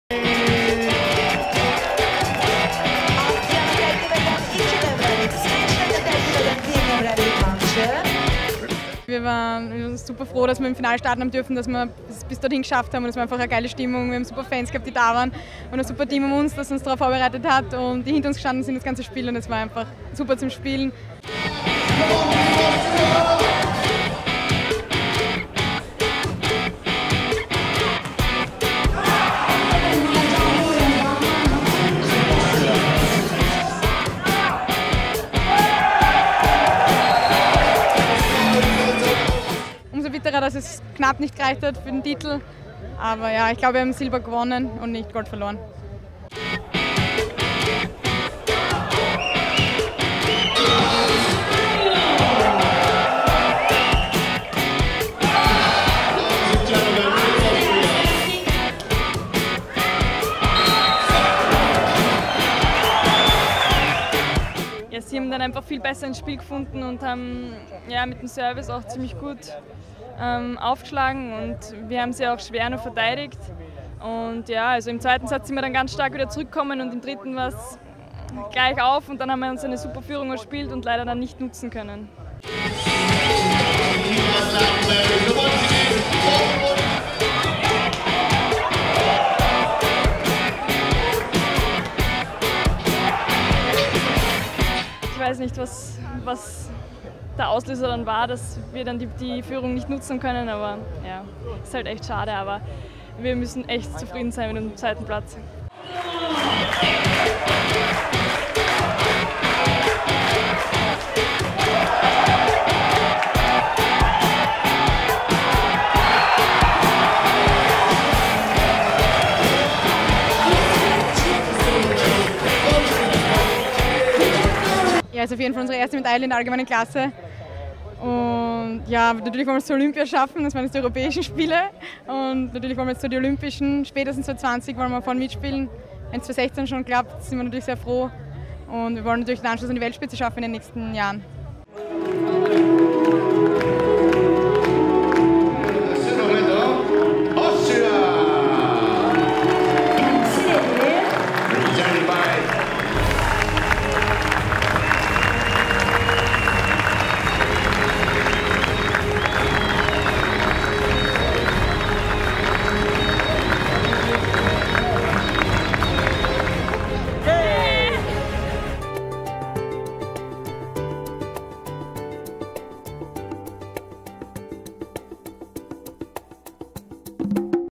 Schützenhofer/Plesiutschnig Beachvolleyball Silbermedaille - Team Austria - Baku 2015
Katharina Schützenhofer und Lena Plesiutschnig nach der denkbar knappen 1:2-Final-Niederlage im Interview

Unser größter Erfolg!"

Wie schlimm ist es, das Finale so knapp zu verlieren?

Lena Plesiutschnig: "Natürlich hätten wir gerne Gold geholt, wir waren auch wirklich sehr knapp dran. Aber am Ende haben die Schweizerinnen sehr stark gespielt. Ihr Erfolg geht in Ordnung."

Was gab am Ende den Ausschlag?

Katharina Schützenhöfer: "Schwer zu sagen. Wir hätten durchaus gewinnen können. Wir haben um jeden Punkt gekämpft. Am Ende hat es nicht gereicht. Aber ich glaube, wir können stolz sein. Die Silbermedaille bei den Europa-Spielen ist unser größter Erfolg bislang. Das stellt unsere Jugend- und Junioren-Erfolge ganz klar in den Schatten."

Lena Plesiutschnig: "Ein Riesen-Dankeschön ans ganze Team. An Leo, Martin, die medizinische Abteilung, ans ÖOC-Büro. Ohne dieses Teamwork würden wir jetzt nicht mit einer Silbermedaille hier sitzen."

Ihr habt in den letzten Tagen sehr viel Videostudium betrieben. Wie würdet Ihr Eure Leistung selbst analysieren?

Katharina Schützenhöfer: "Die Erfolge gegen die favorisierten Teams aus den Niederlanden und Russland geben uns sehr viel. Wir haben in den acht Spielen wiederholt bewiesen, dass wir sehr gut Volleyball spielen können. Woran wir ganz klar arbeiten müssen, ist unsere Konstanz."

Wie werdet Ihr Silber feiern?

Lena Plesiutschnig: "Wir sind nach den acht Spielen verdammt müde, brauchen erstmals Ruhe. Aber spätestens in Wien wird gefeiert."

Wie geht's für Euch weiter?

Katharina Schützenhöfer: "Wir trainieren die nächsten zwei Wochen in Wien, dann spielen wir ein Challenger in Vaduz. Dann steht schon die Heim-EM in Klagenfurt vor der Tür. "